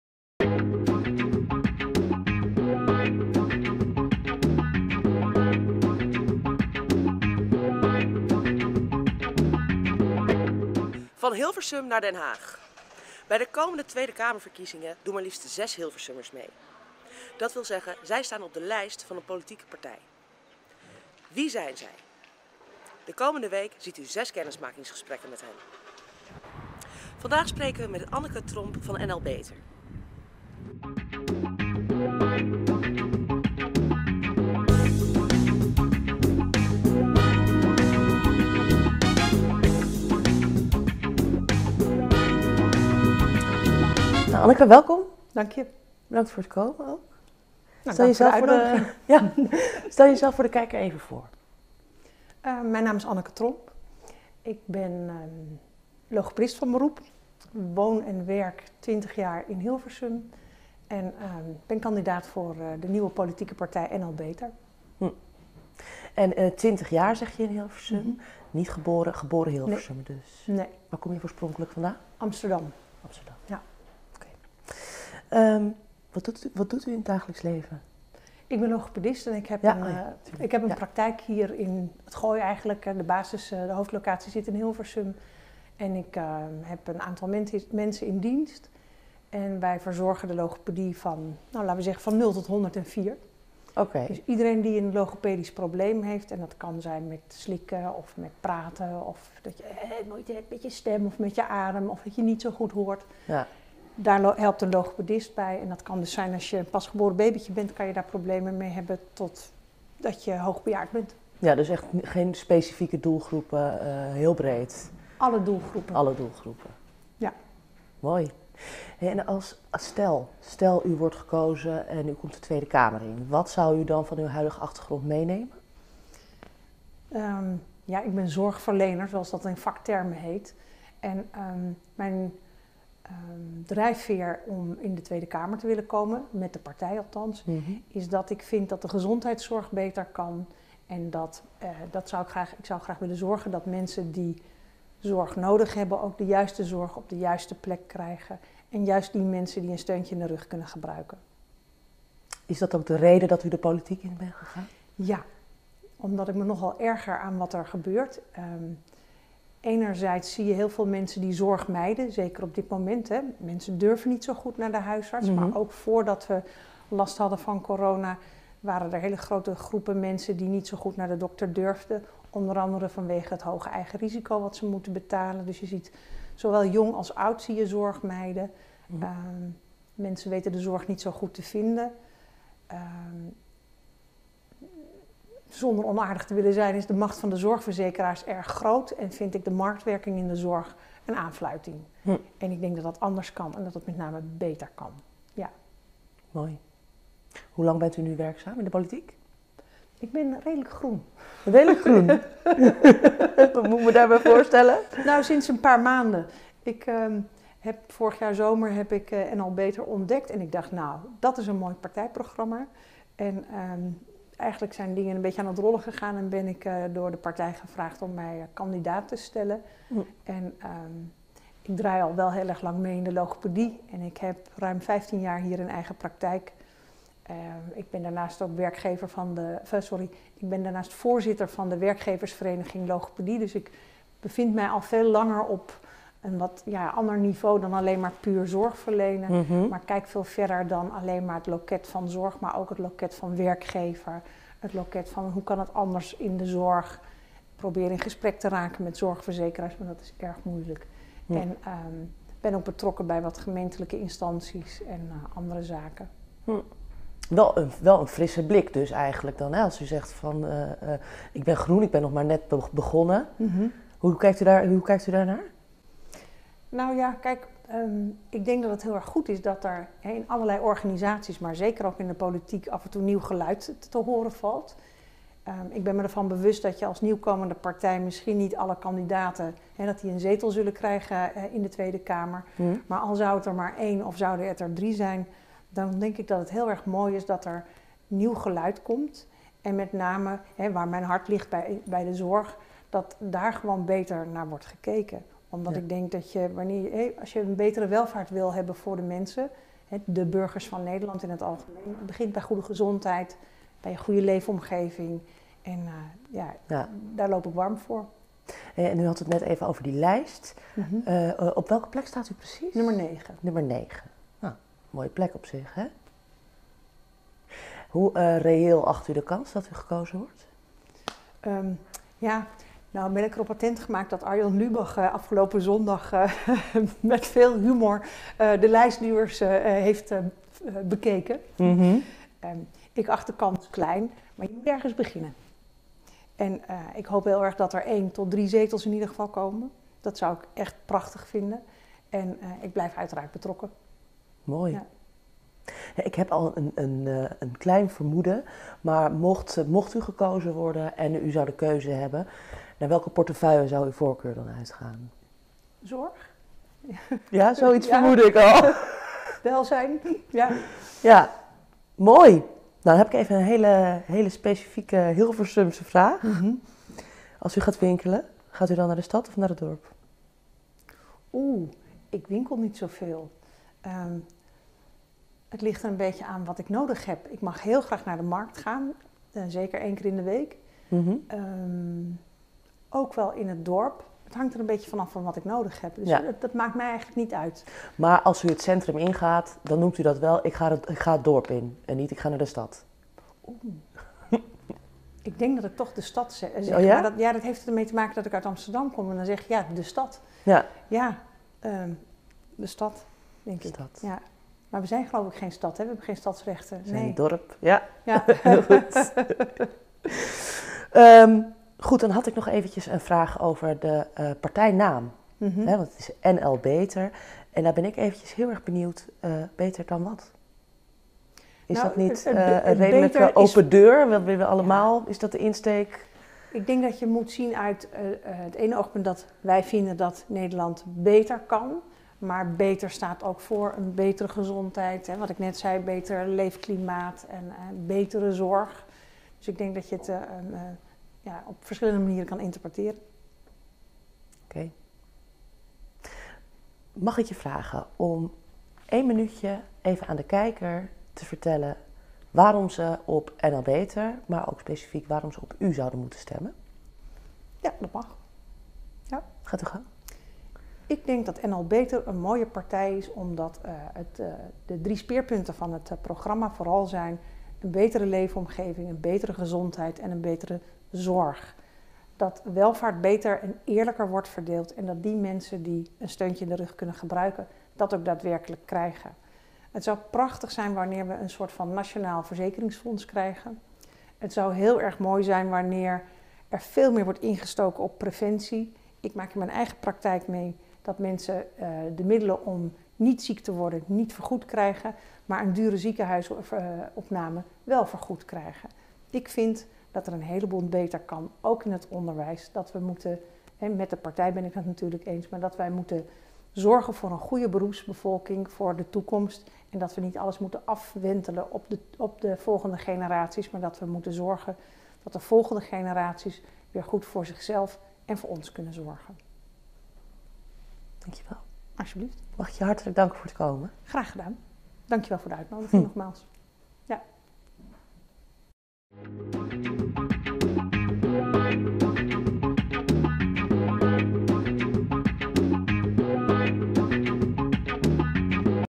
Van Hilversum naar Den Haag- aflevering 3- Anneke Tromp
In de serie: “Van Hilversum naar Den Haag” maken we kennis met 6 Hilversummers die op de kandidatenlijst staan voor hun politieke partij voor de 2e Kamerverkiezingen. Wie zijn zij en wat beweegt ze? In de 3e aflevering praten we met Anneke Tromp van de partij NL Beter. Presentatie: Savannah Stolk. Productie en regie: Boy Scholte. Met dank aan Hilversummers.nl, Allard Bentvelsen en Benji van Geene. Dit programma werd geproduceerd door Politiek Debat Hilversum in samenwerking met Hilversummers.nl.